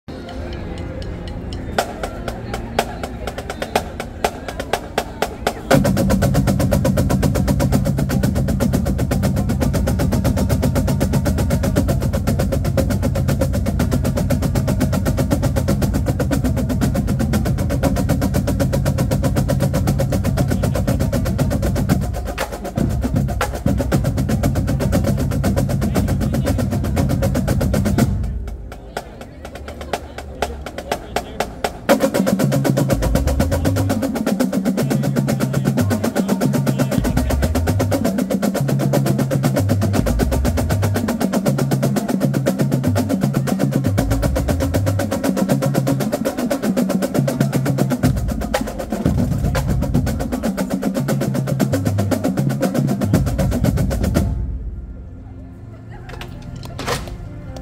Stick Control - 9/28/24 - Kennesaw State University Drumline
KSU Drumline 2024
Warm-up Before University of Tennessee at Martin (UT Martin) Football Game
KSU Bandsmart